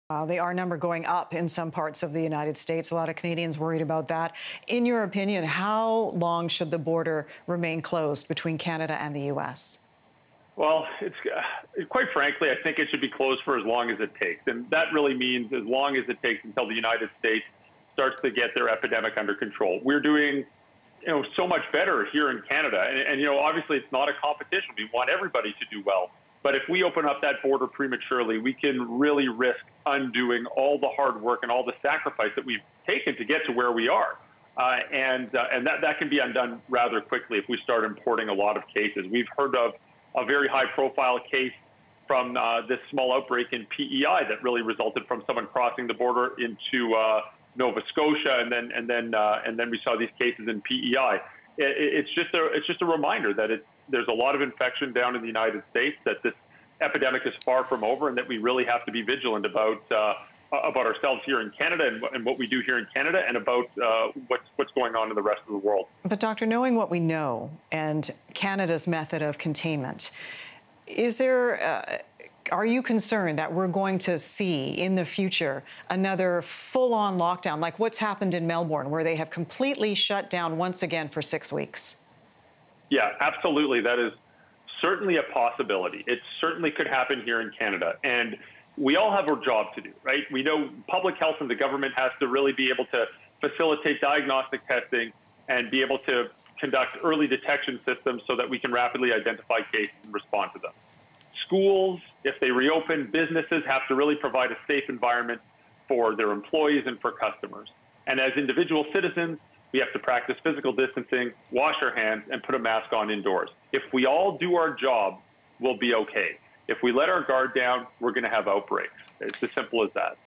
COVID-19 is so bad in the U.S. that opening the border could spark new outbreaks in Canada: expert
Infectious Disease Expert Isaac Bogoch says that the border needs to remained closed 'for as long as it takes' until the U.S. starts to better control the spread of COVID-19 and opening it too soon could spark new outbreaks in Canada.